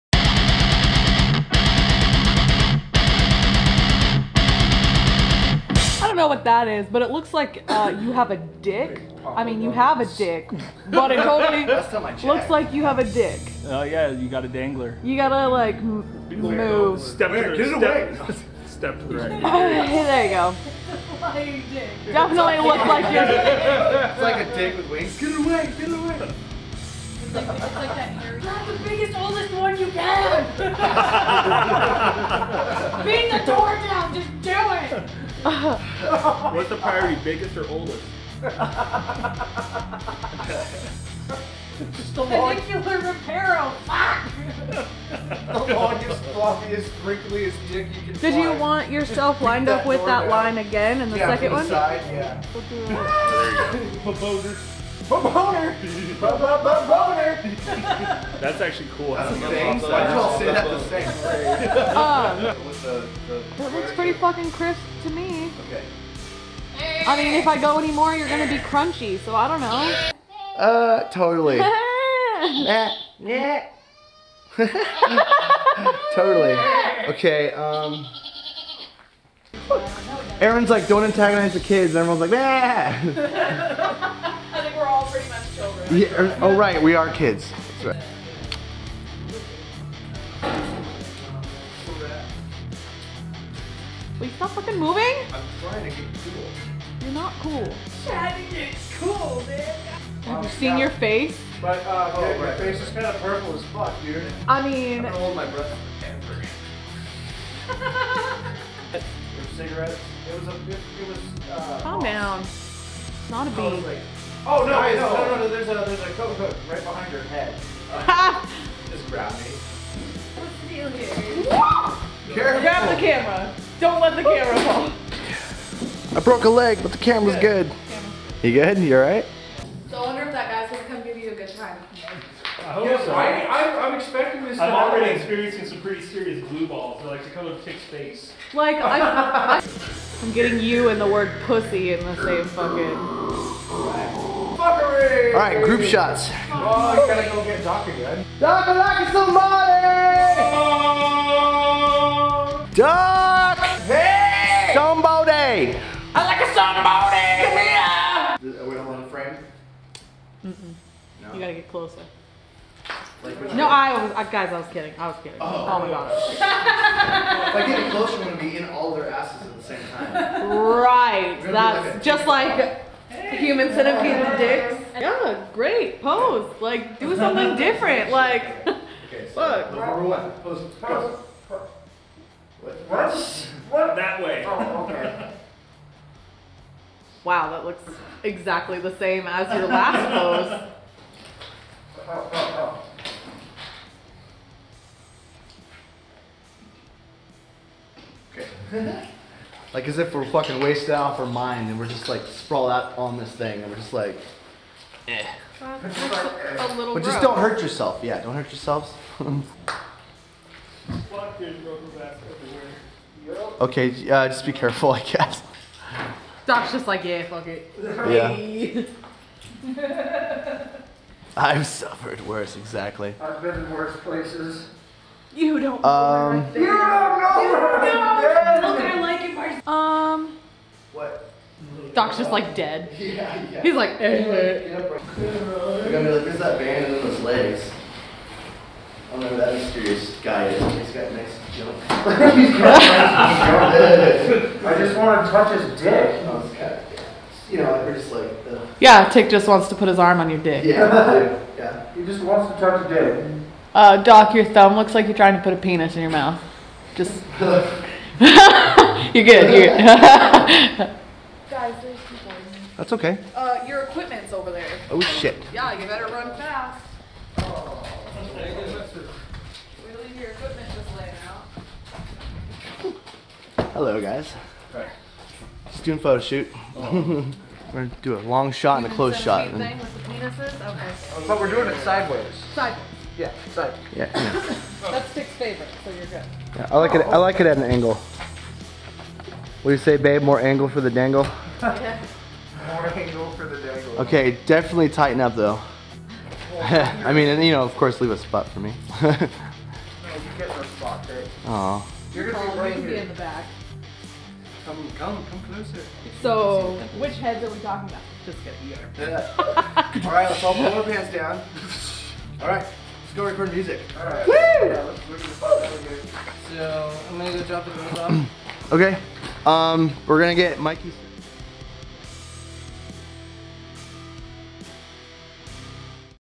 Behind the screams Photoshoot - Part 3 - Cryptamnesia
When we first got together to take our photos while Doc was our lead guitarist. Shot at the Northern State Asylum Barns.